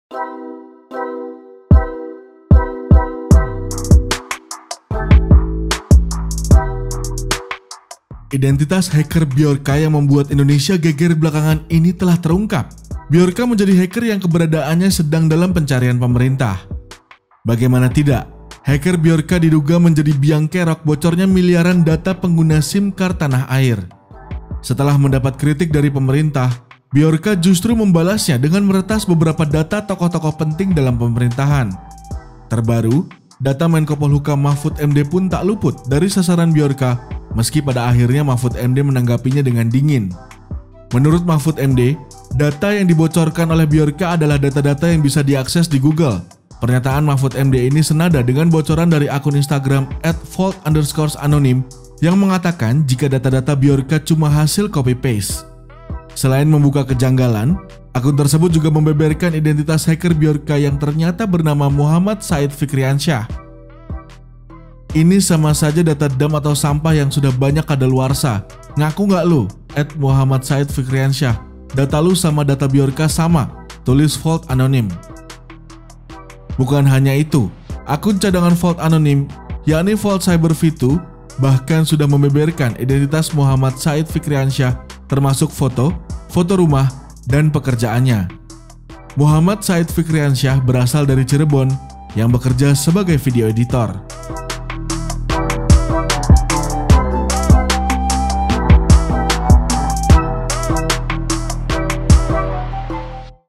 Identitas Bjorka Terungkap? Ternyata Bocah Cirebon?
Bjorka menjadi hacker yang membuat pemerintah resah belakangan ini. Apalagi, Bjorka cukup sering menunjukkan bahwa dia berhasil membobol data-data orang pemerintahan.